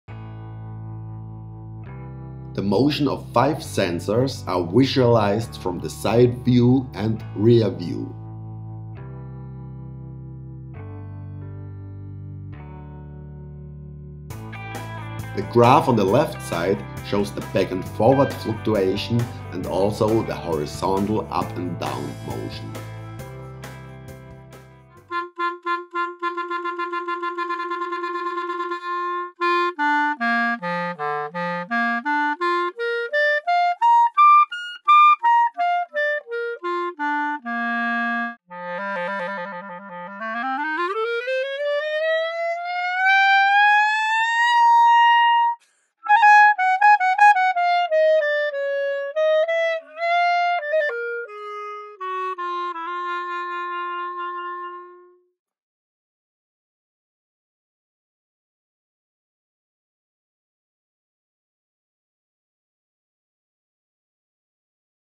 Clarinet Playing | 3D Tongue Motion Visualization in Realtime
Watch the tongue motions of while clarinet playing.: A 3D Motion Analysis of Clarinet Tonguing recorded by Articulograph AG 501 (Carstens Medizintechnik)

by Dr. Matthias Bertsch (University of Music and Performing Arts, Vienna) in Cooperation with PD Dr. Phil Hoole (Ludwig-Maximilians-University Munich.
Performer: Julia Parzer. Recorded with 250 images per second, played with 30 fps. (1 sec=8,3 sec).